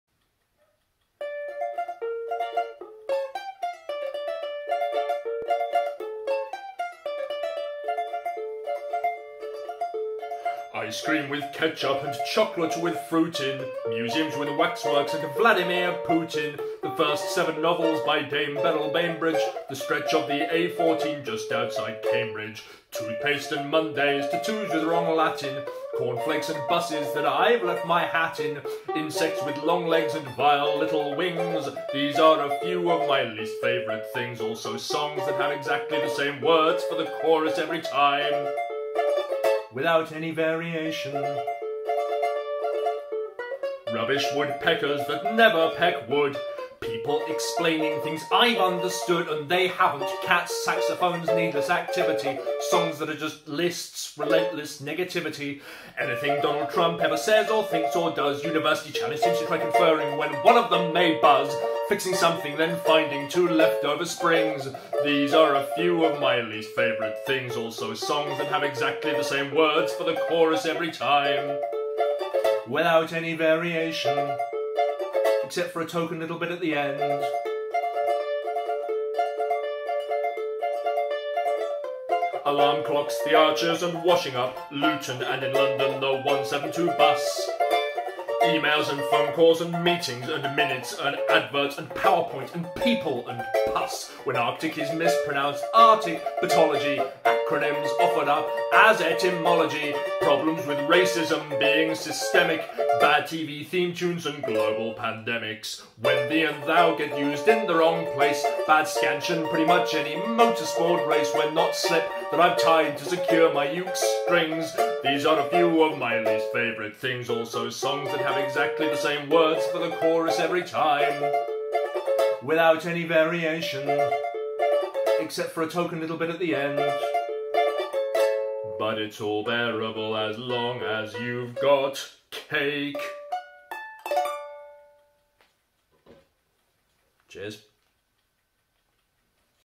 The Roses Have Got Mildew, And The Kettles Need Polishing Again
It occurred to me that what the world really needs, in these uncertain and unsettling times, is more footage of a middle aged bloke whinging about trivial nonsense. You're welcome.